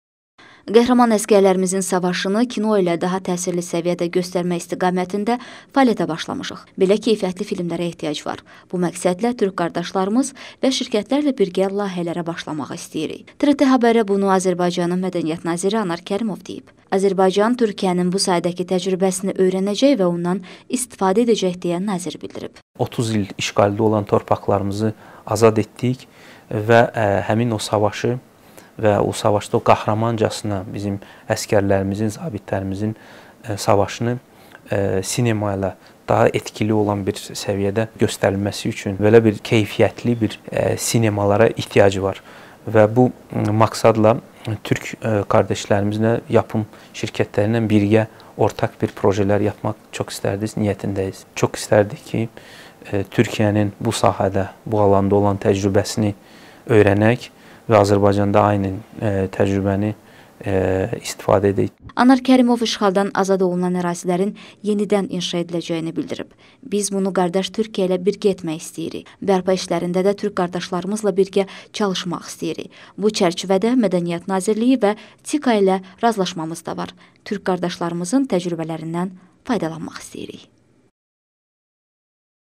AZƏRBAYCAN VƏ TÜRKİYƏ KİNO SAHƏSİNDƏ İŞ BİRLİYİNƏ HAZIRLAŞIR
Türkiyə və Azərbaycan Qarabağ müharibəsi haqqında film çəkəcək